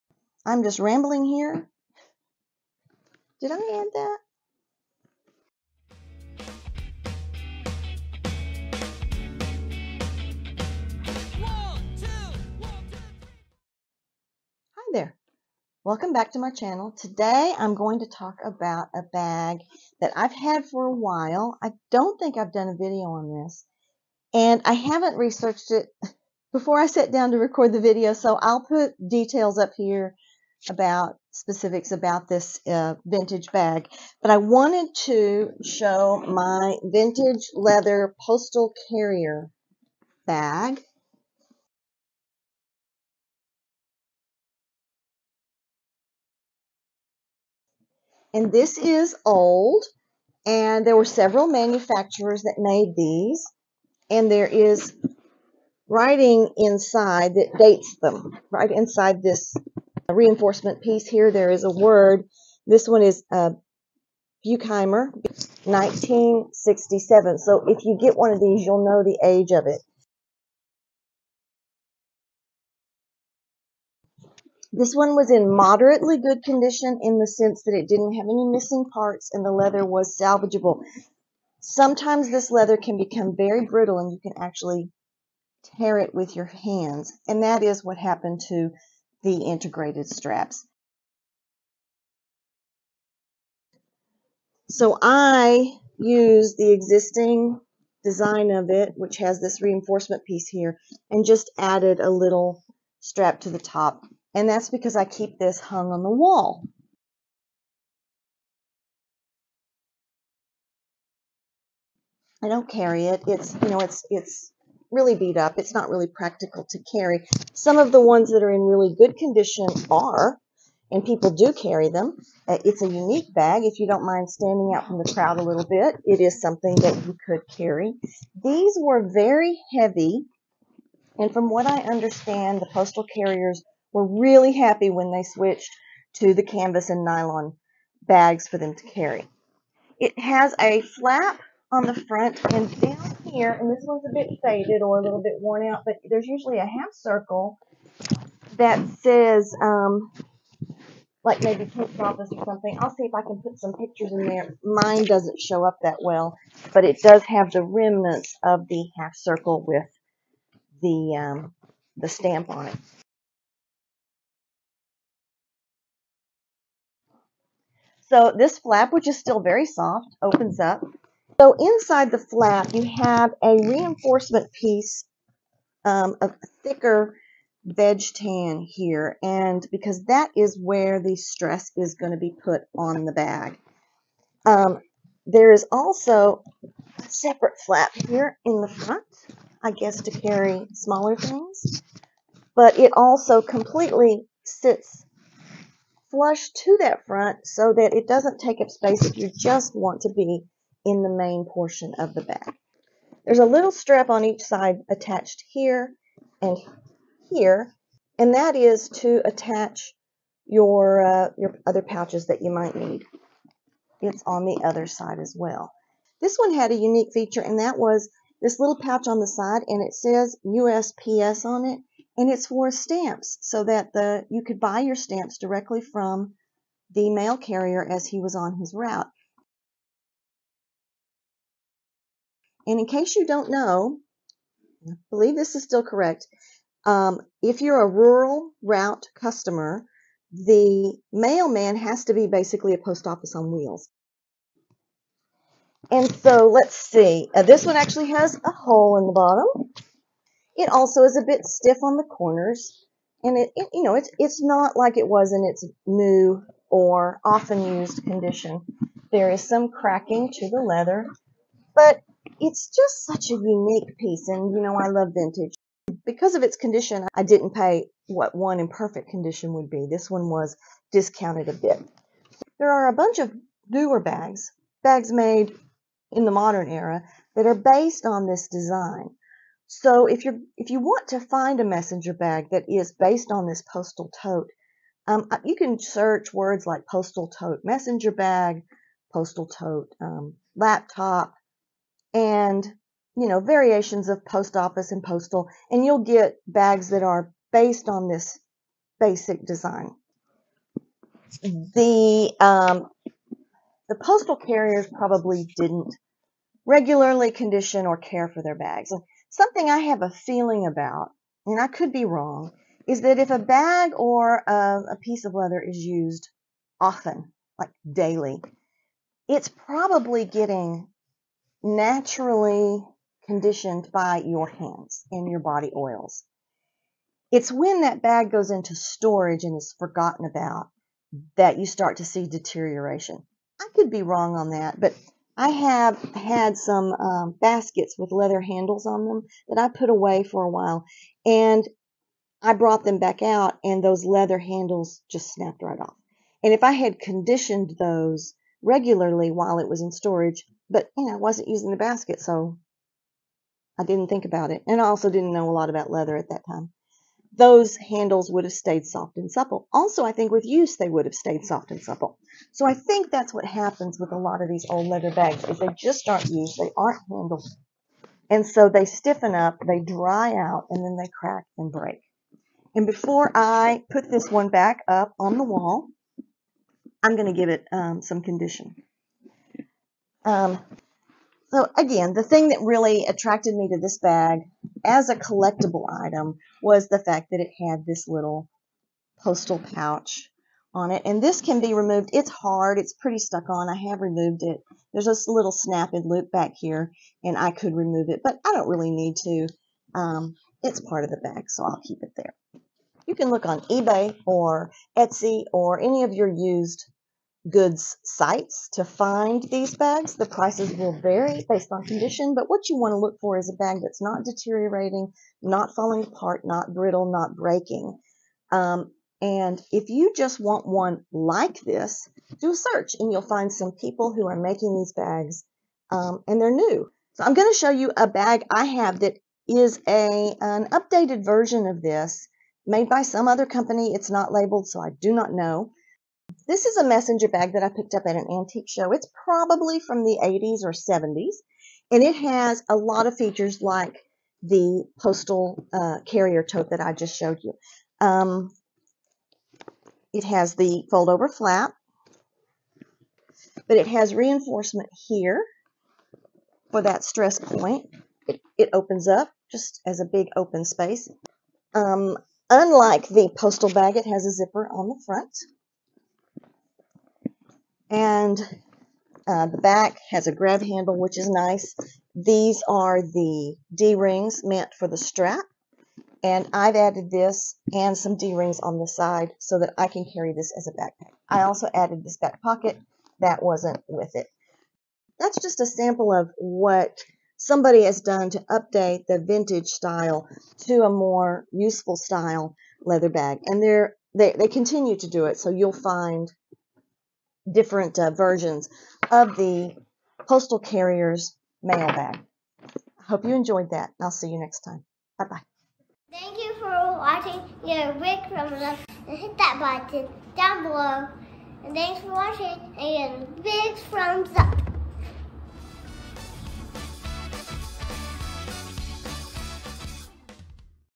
Vintage Postal Mail Bag History and overview Let's go Postal
Let's take a look at a piece of history with this vintage Leather Postal Mail Bag. I'll give some history and up close looks of one of my favorite finds. It's both Vintage and Leather, two things I enjoy the most.

Links:
History of the Leather Postal bag

Margo’s Etsy Shop